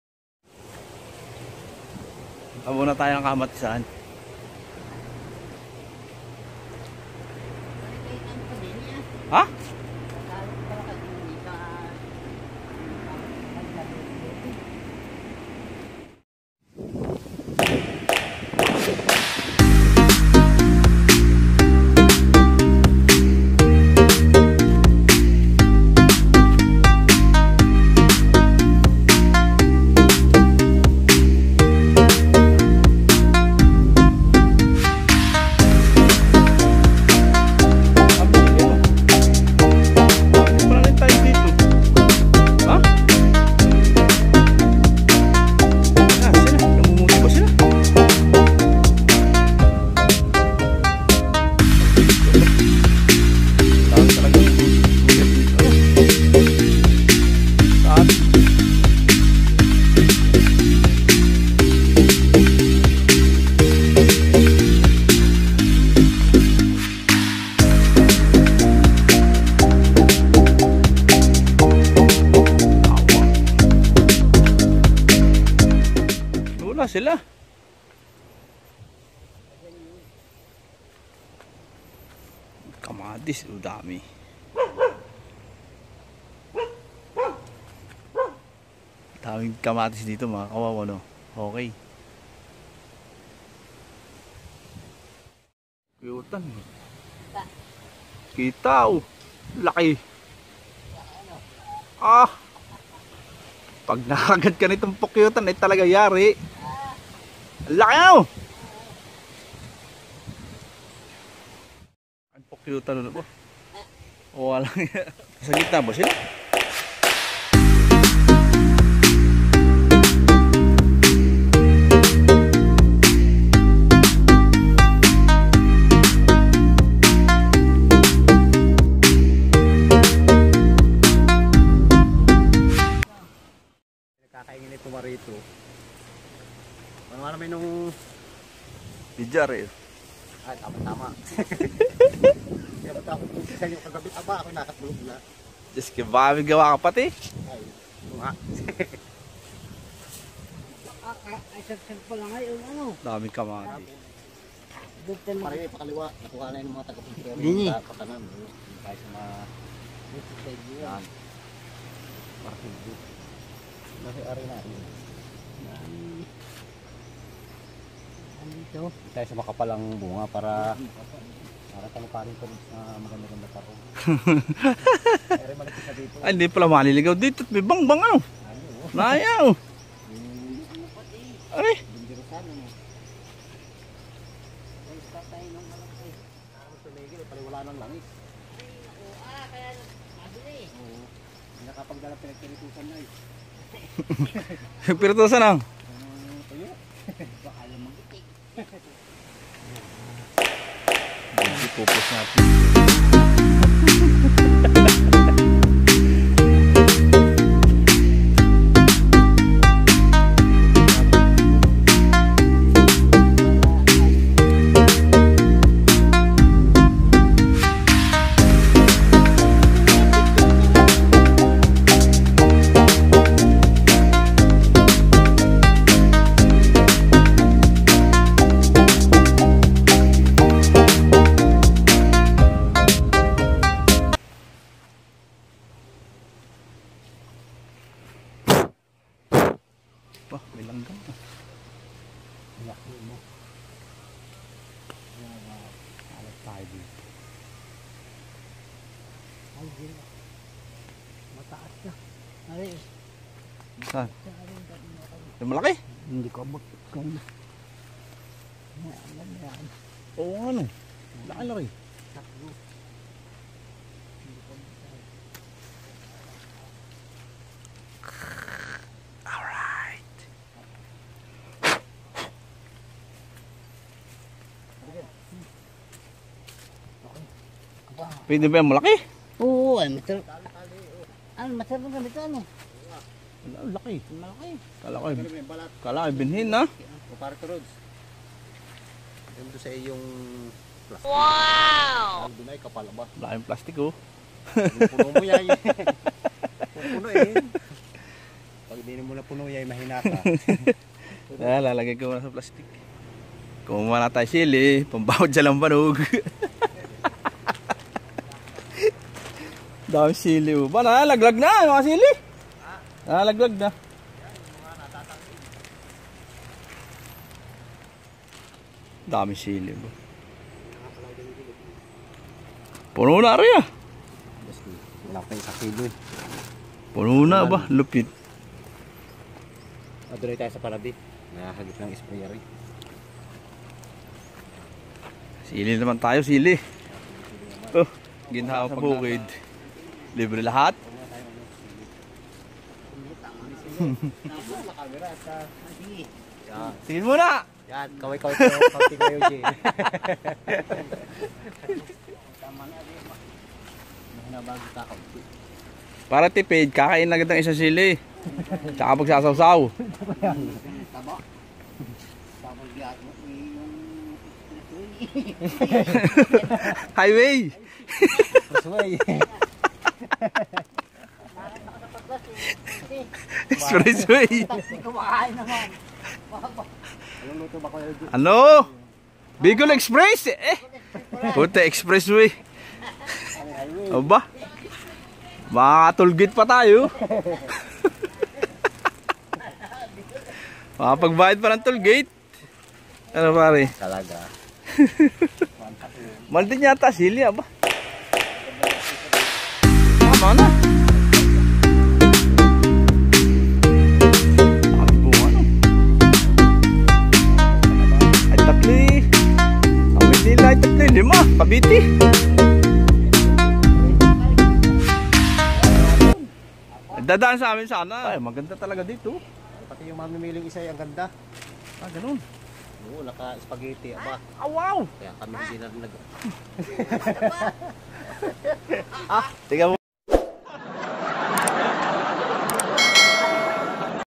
BUHAY PROBINSYA.MAGLULUTO DAW.NANG EXPRESS WAY.BIKOL EXPRESS PALA HAHAHA.Salamat paring JOSEPH
This is for entertainment only.And thank u paring JOSEPH and KUYA EUGIN